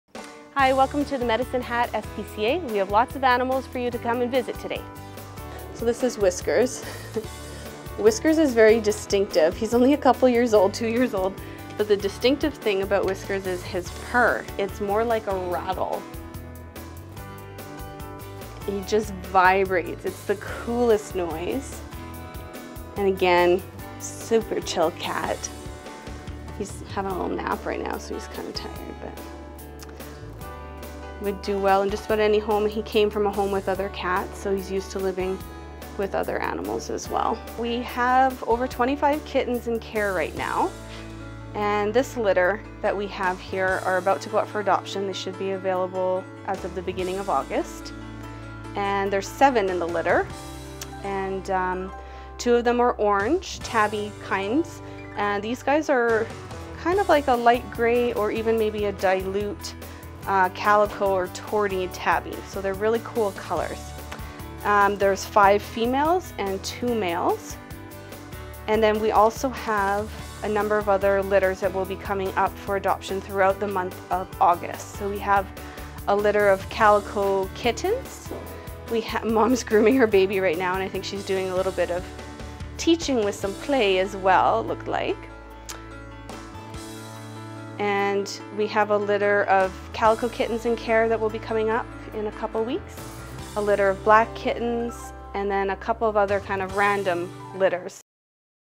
SPCA - cats needing to be adopted
There are several cats and kittens looking for new homes at Medicine Hat SPCA.